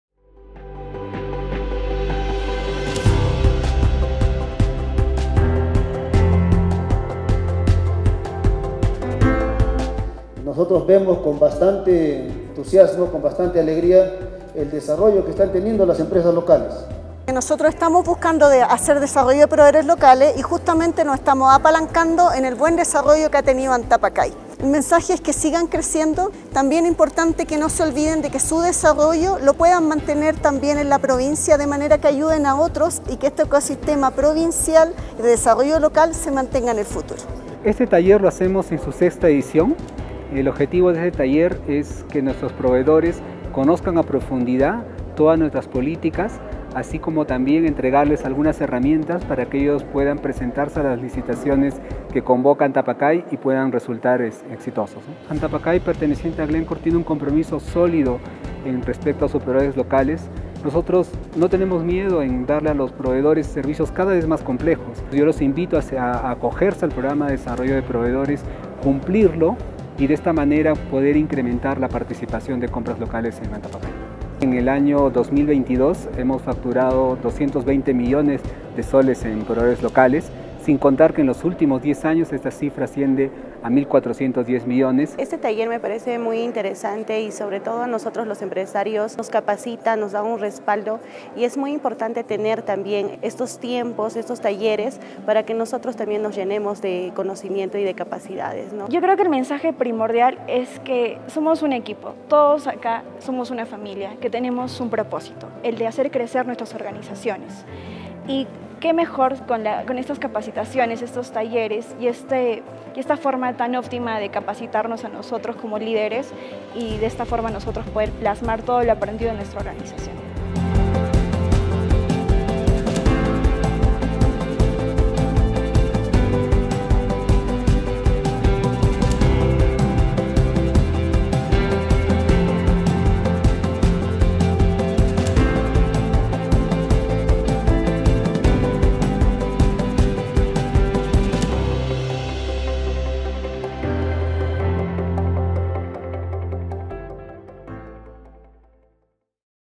BRÚJULA SOCIAL l VI TALLER ABC DEL CONTRATISTA 2023
¡Crecimiento conjunto! Gracias a nuestro Programa de Desarrollo de Proveedores Locales, más empresas de Espinar están brindando servicios a @Antapaccay. ¡Economía en acción! 💼🏭
De 42 a 129 empresas en 2022, generando 220 millones de soles en 2022 y 1470 millones en 10 años. ¡Continuamos en marcha! 👷‍♀